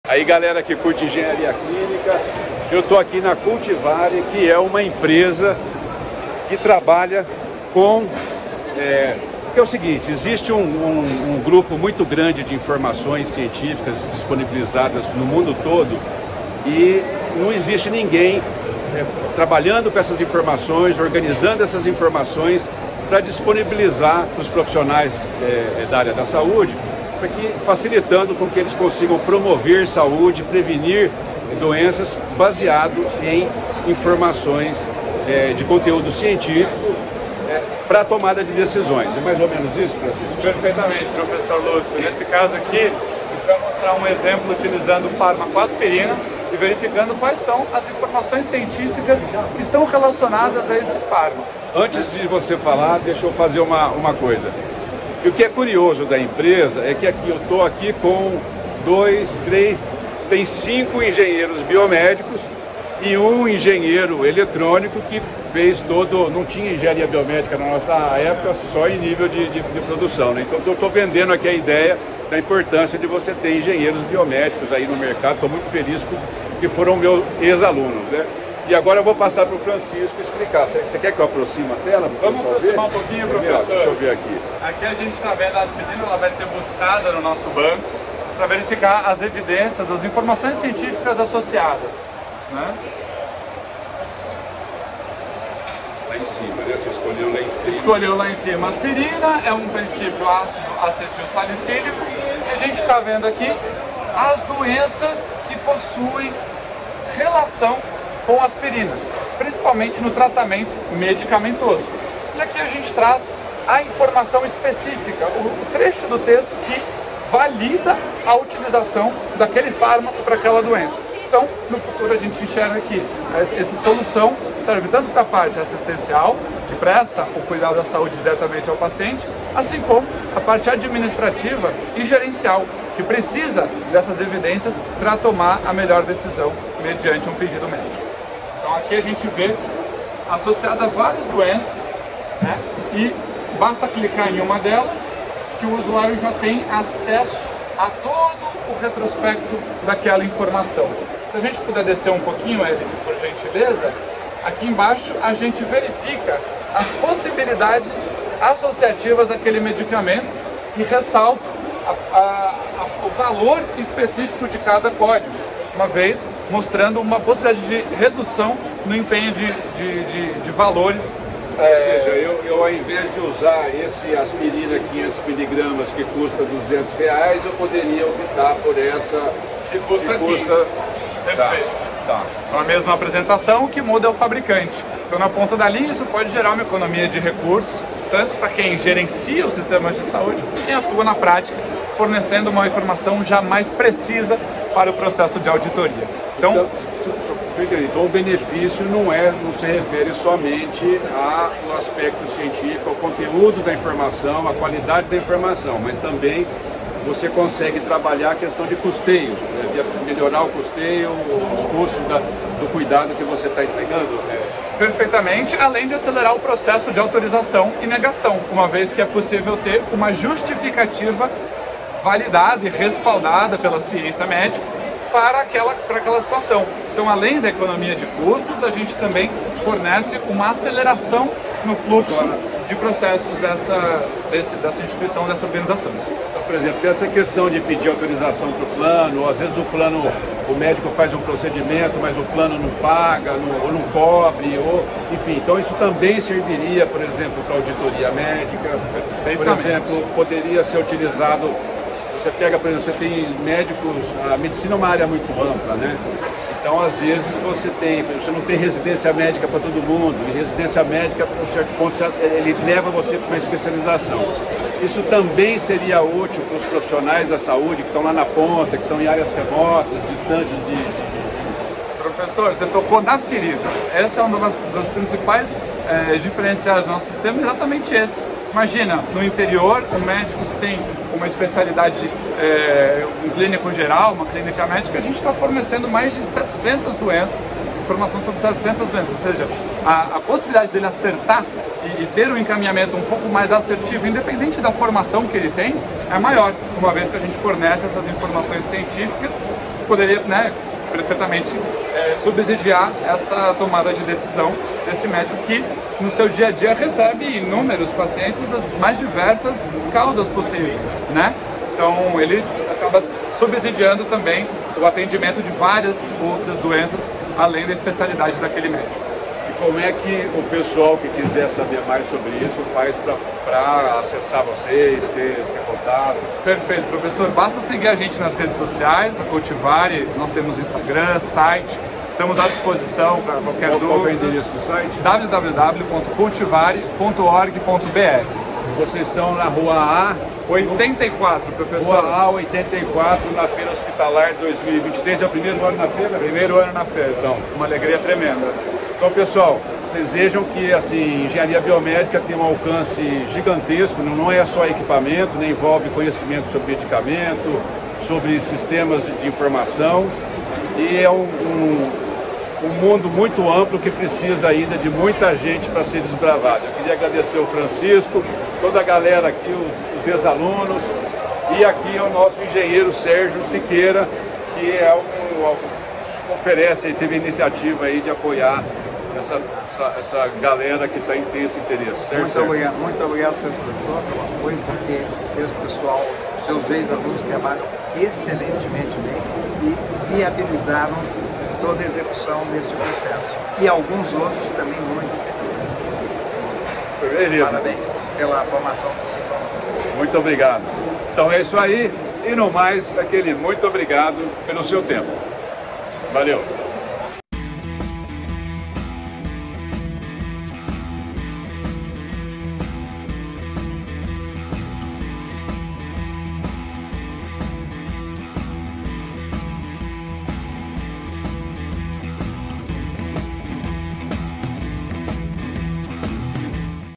Engenharia Biomédica Cultivare: tratar dados, prevenir doenças e promover saúde na Hospitalar 2023
A engenharia biomédica da Cultivare tem por objetivo tratar dados para prevenir doenças e promover saúde. Eles utilizam um enorme conjunto de evidências científicas na área da saúde para oferecer, de forma organizada, o acesso a informação para tomadas de decisões, tanto de natureza médica como econômica. Deste modo, ajudam hospitais, profissionais da saúde, planos e operadoras de saúde, ensino e empresas de auditoria médica.
CULTIVARE
𝐂𝐨𝐧𝐬𝐢𝐝𝐞𝐫𝐞 𝐚𝐩𝐨𝐢𝐚𝐫 𝐨 𝐧𝐨𝐬𝐬𝐨 𝐜𝐚𝐧𝐚𝐥.